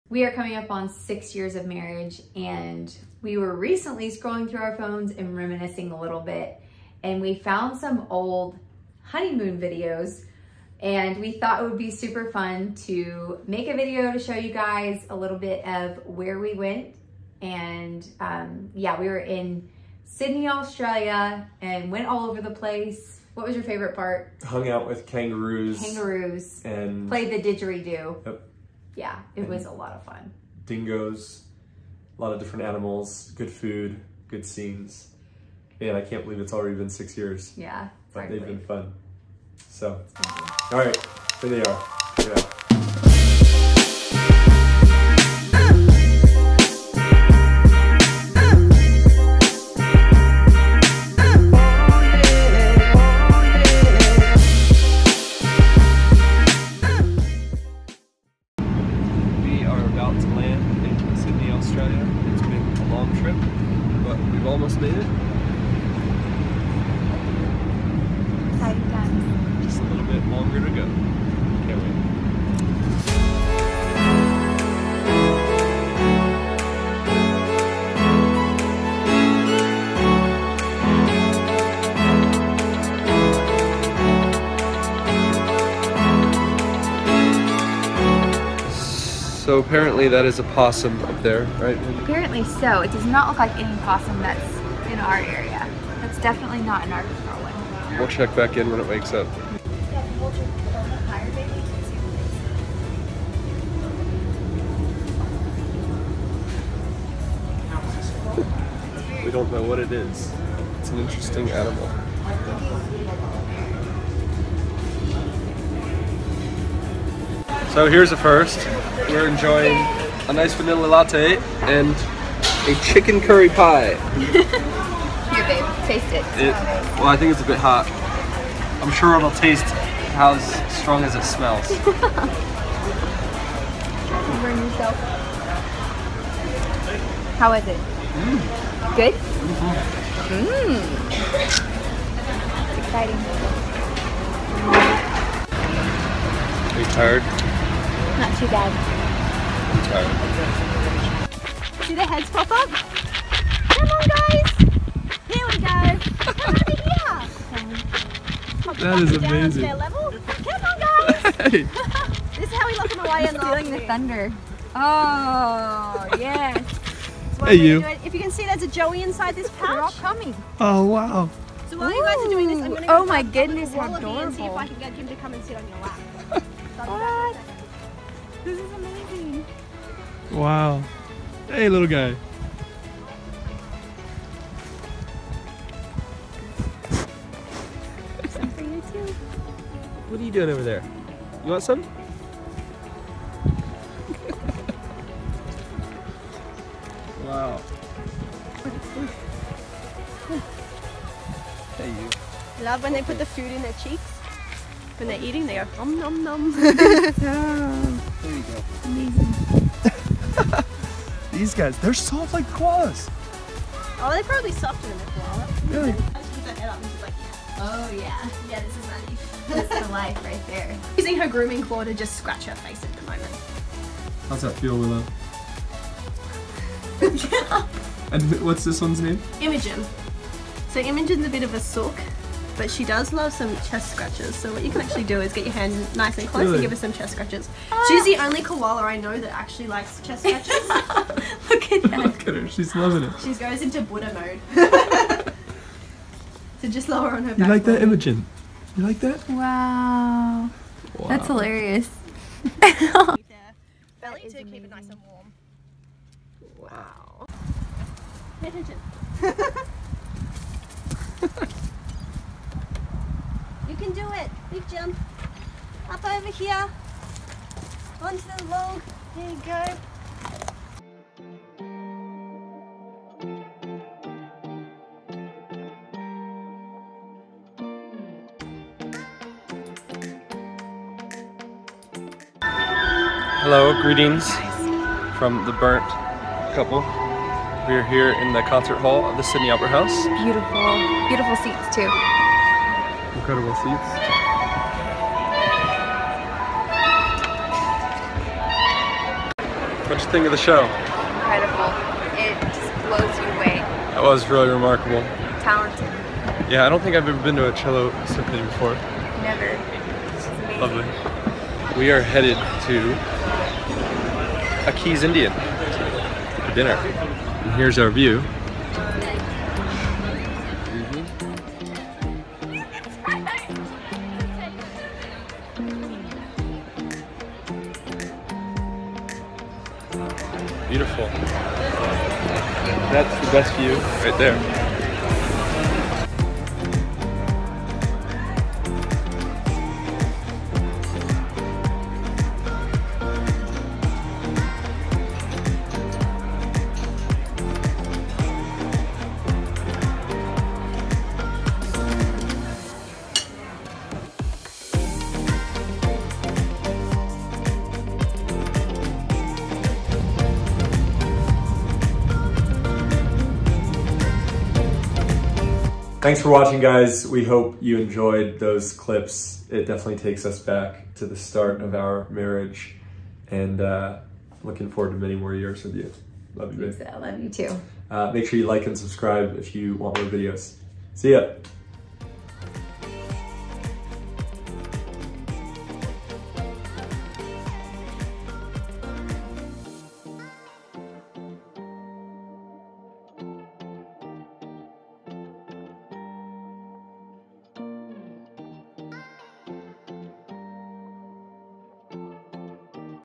OUR HONEYMOON IN AUSTRALIA
As we approach six years of married life, we were doing some reminiscing and dug up some clips from our honeymoon in Australia!